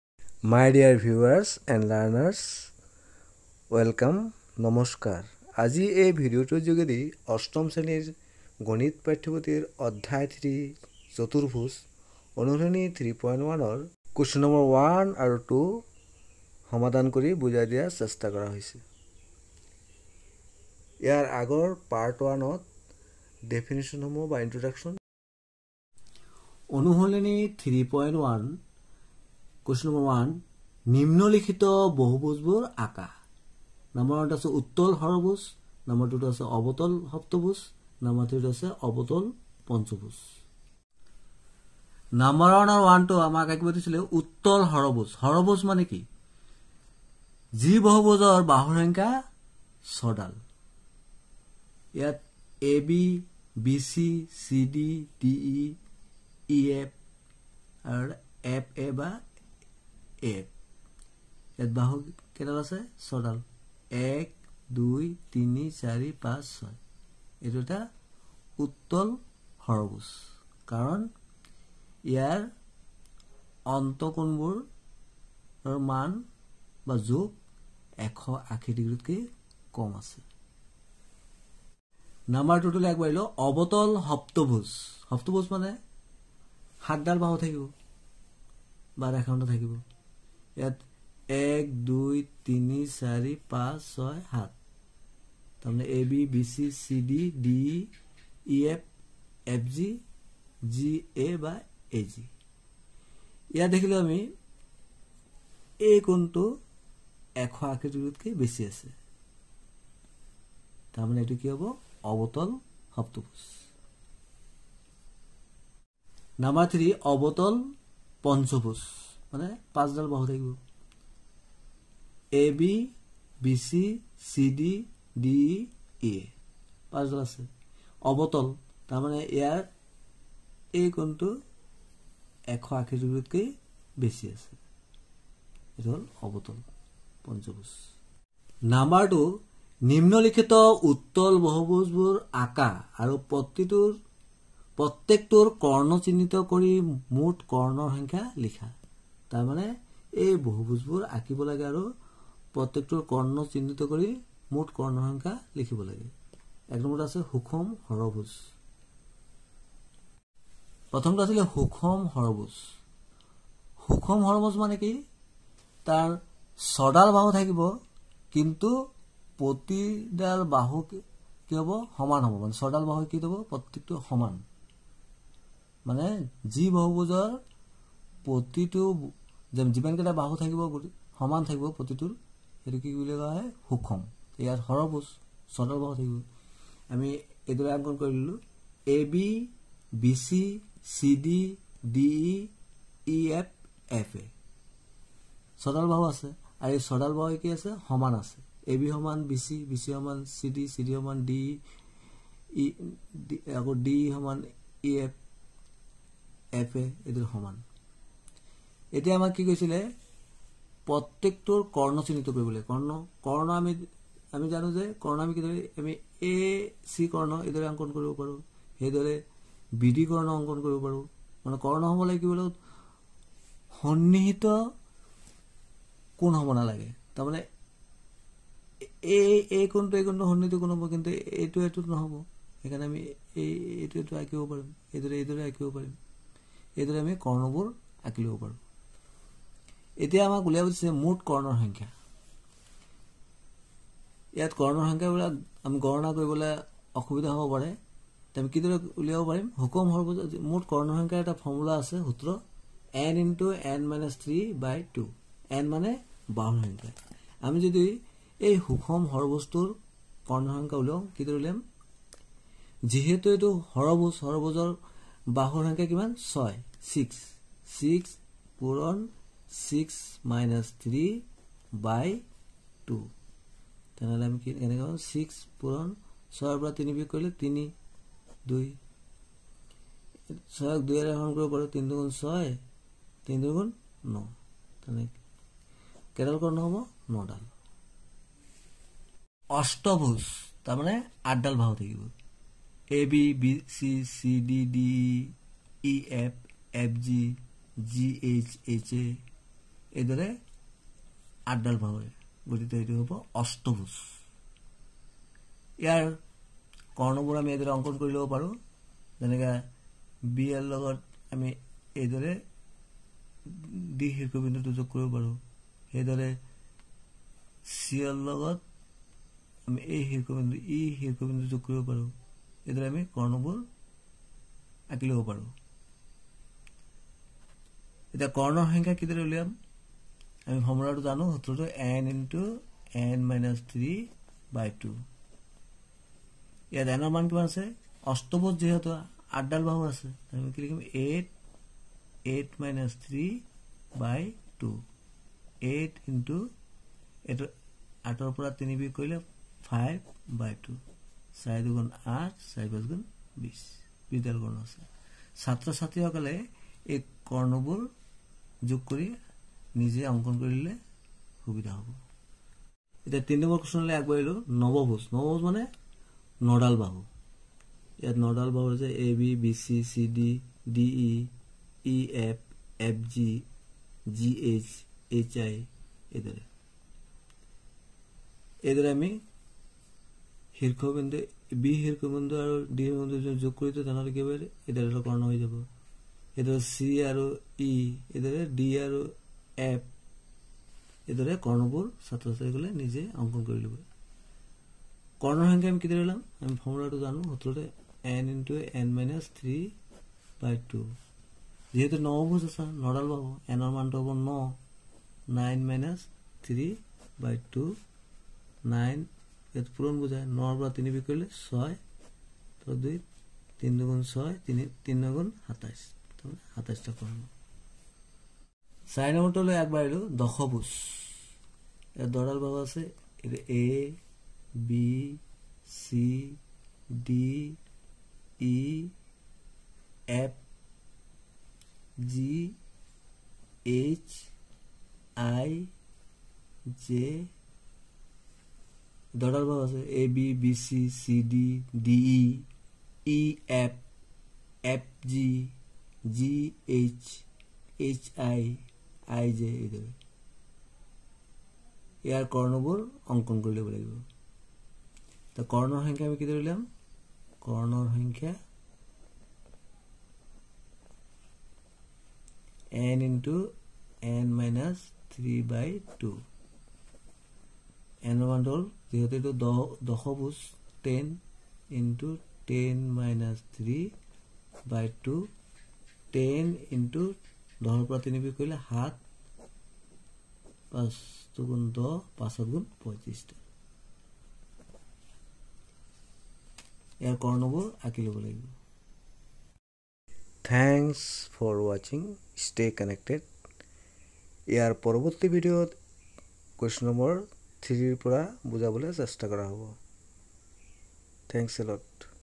CLASS-8 MATHS, CHAPTER-3, EXERCISE: 3.1, Q. NO. 1 & 2 , PART-2 (SCERT,ASSAM)
বহুভুজ অংকণ আৰু বহুভুজৰ কৰ্ণ সংখ্যা  নিৰ্ণয়